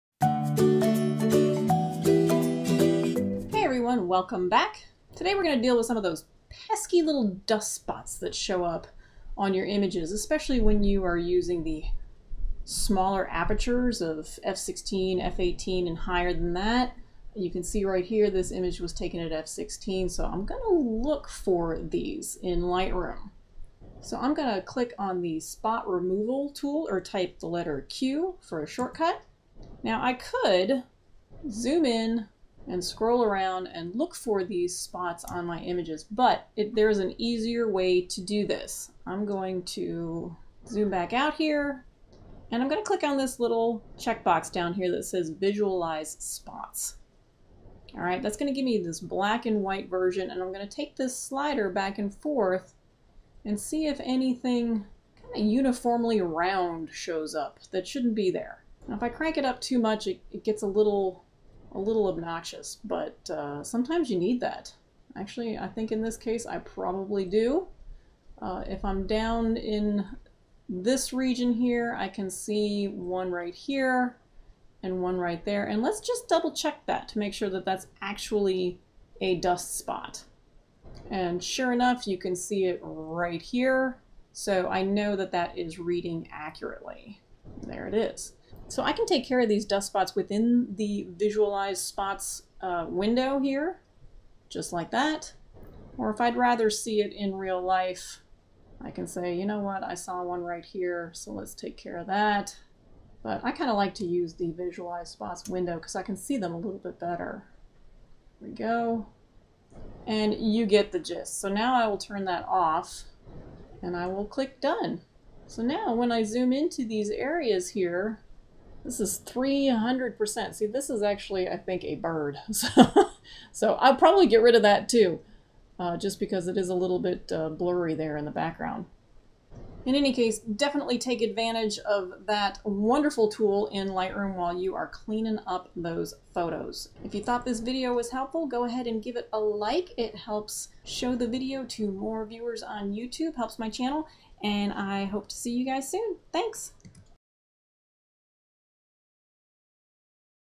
How to Remove Dust Spots In Lightroom Classic
Learn how to use the visualize spots tool to easily remove all the dust spots on your images in Lightroom Classic.
for workshops and on-line classes